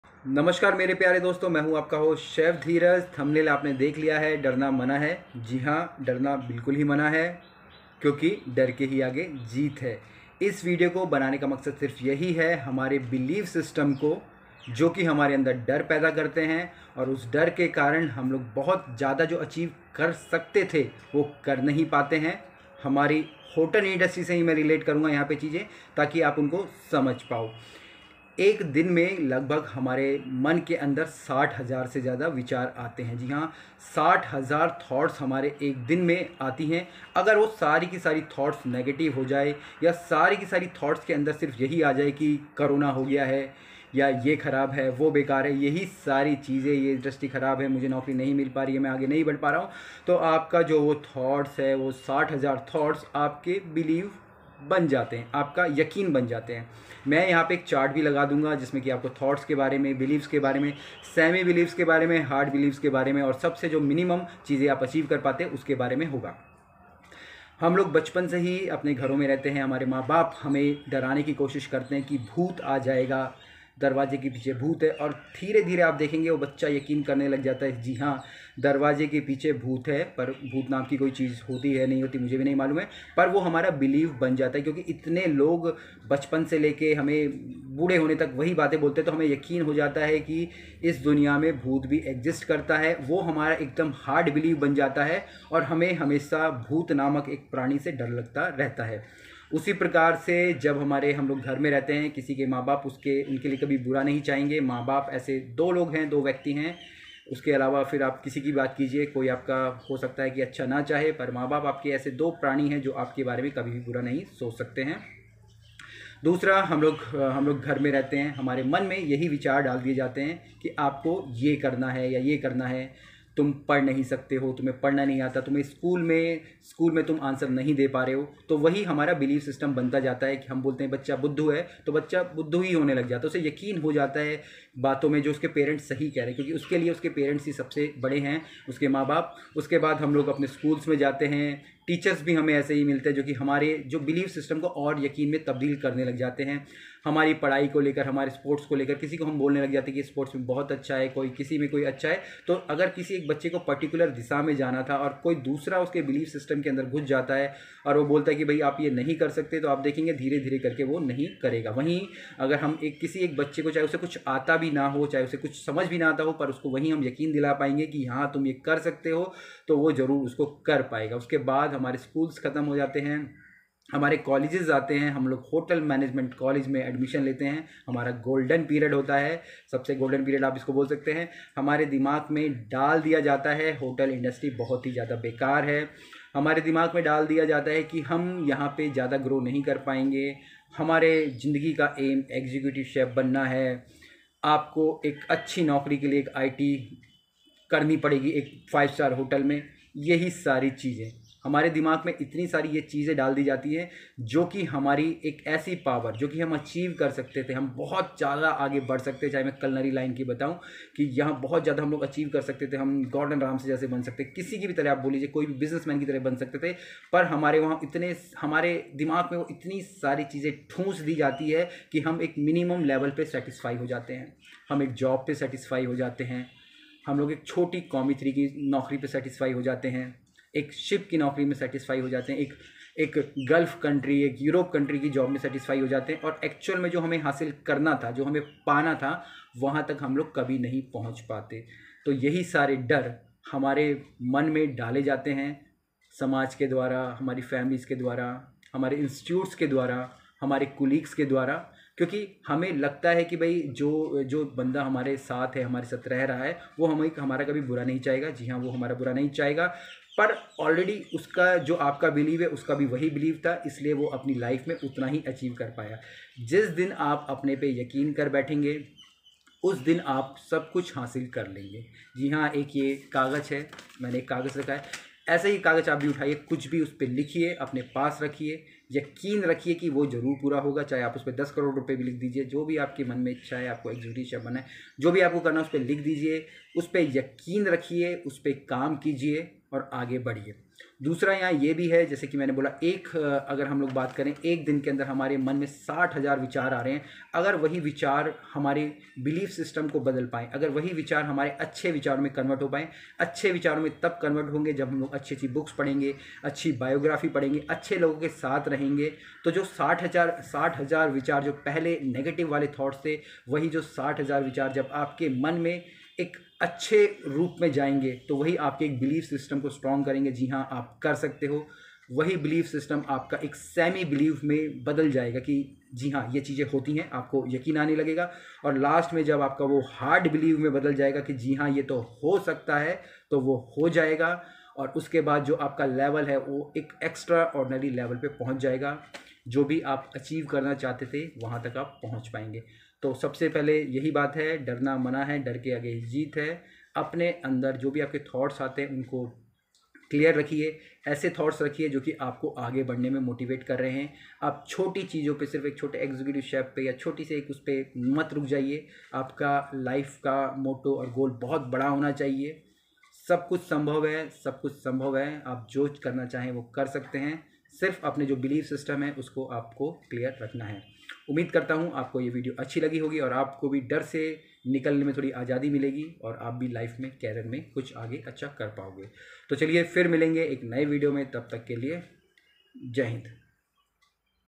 Be Fearless 🙄 !! What is Belief System !! How To Achive Massaive Sucess 🌞 in Life !! Be Yourself
in This video we are Discussing about Belife system of Human & How we can Achieve sucess.